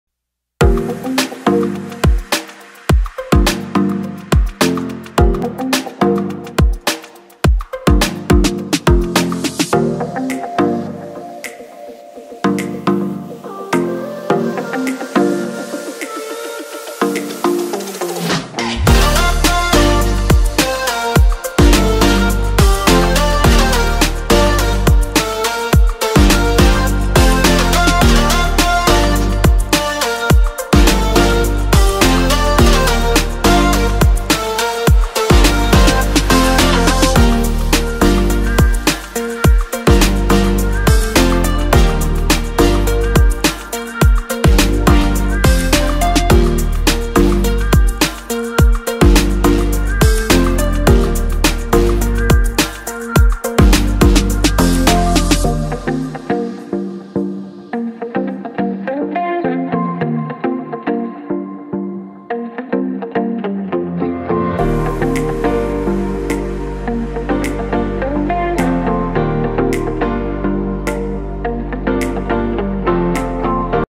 BOHO ART || EASY || ABSTRACT ART || BY SWARA
Boho Art
This type of art is easy to make and you can also use it as a wall art.